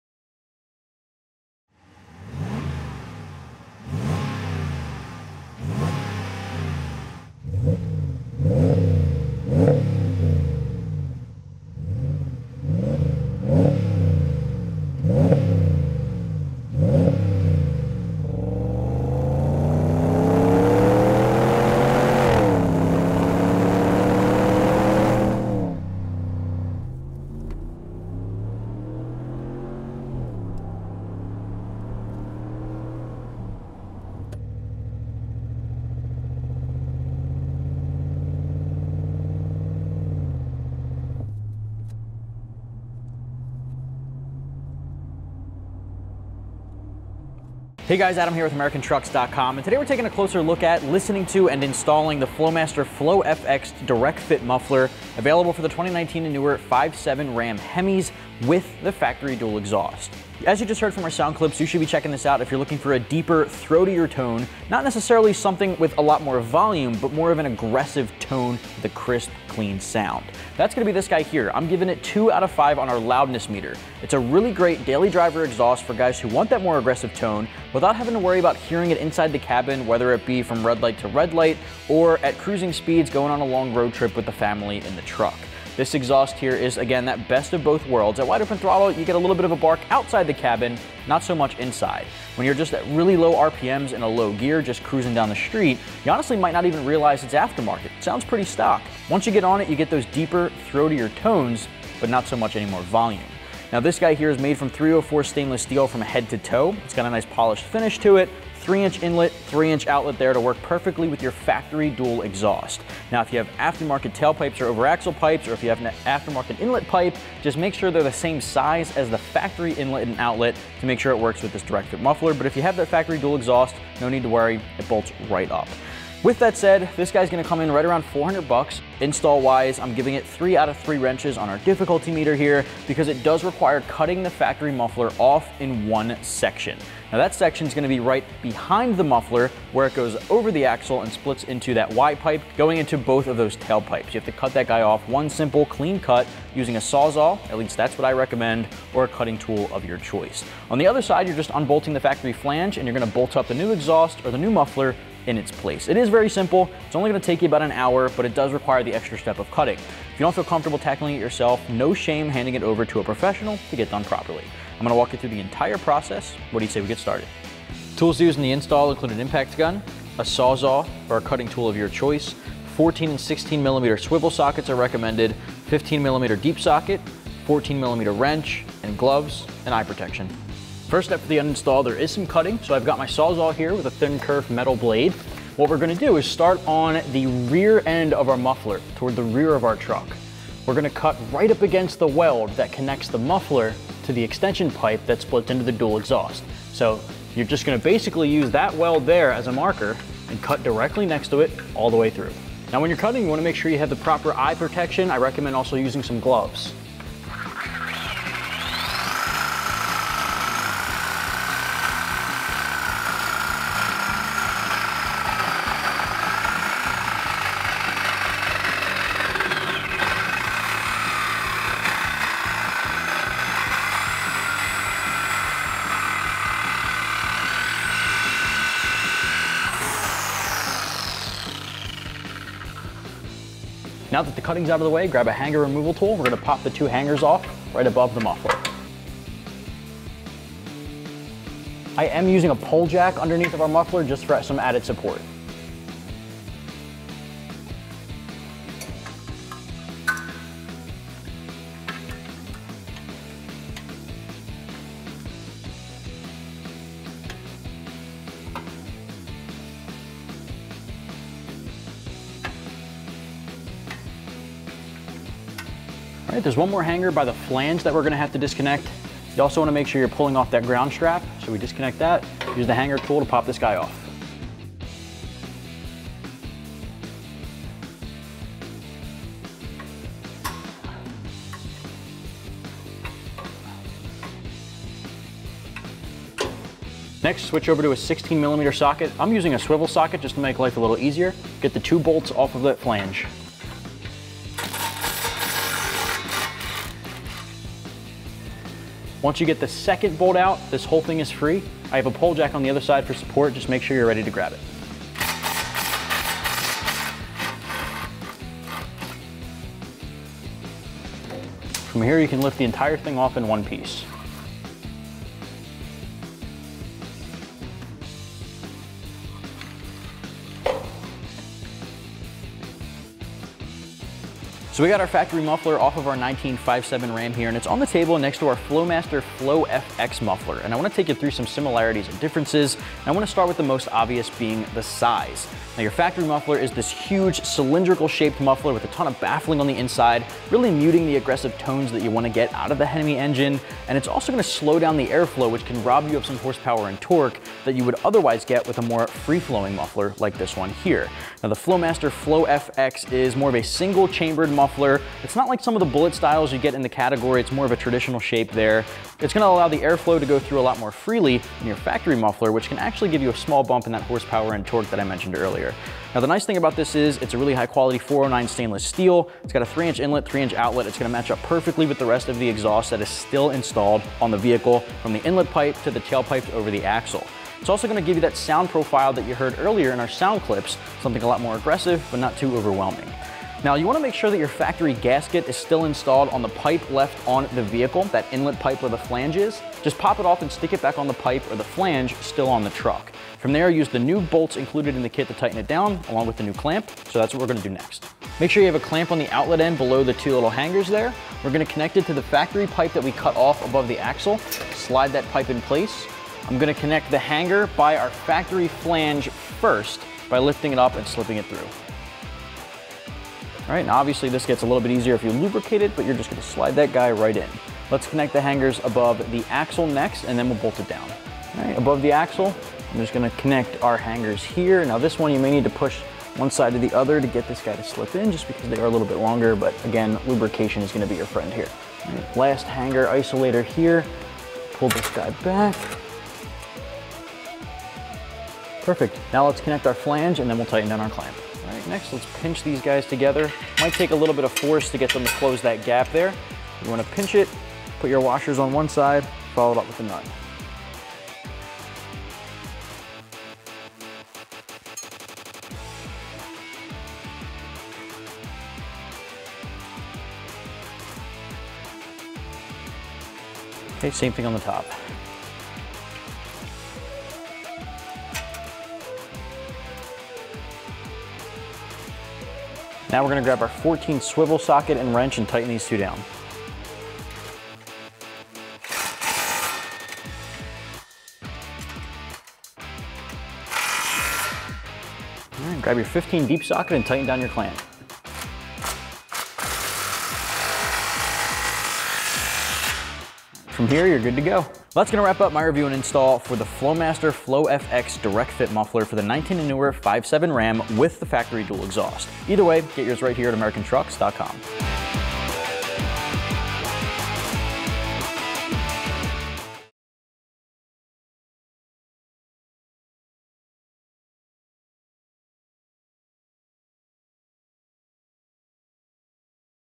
2019-2021 RAM 1500 5.7L Flowmaster FlowFX Direct-Fit Muffler Sound Clip & Review
Upgrade your RAM’s exhaust tone with the Flowmaster FlowFX Direct-Fit Muffler. This high-performance muffler transforms your truck’s exhaust tone, from its standard truck sound into a deep muscular growl from idling to acceleration. As loud as its exhaust sound gets however, the interior noise provided by this muffler never gets out of hand, ensuring a comfortable ride. Additionally, this muffler comes with a straight-through design that ensures optimal exhaust efficiency for maximum performance.

Sturdy Build Quality. The Flowmaster FlowFX Direct-Fit Muffler is constructed from 409S stainless steel for superior strength and durability. This muffler features 3-inch wide tubing, with a straight inlet, and a mandrel-bent off-set outlet tube. Joints are fully-welded for added rigidity. Lastly, this muffler assembly comes in a handsome brushed stainless steel finish.

Direct Installation. The Flowmaster FlowFX Direct-Fit Muffler is designed as a direct bolt-on assembly, utilizing the factory hangers and couplings. Some cutting to the factory piping is required for installation.

AmericanTrucks.com is your home for everything RAM We offer the top mods, price-matching, and have a team of the best RAM experts around.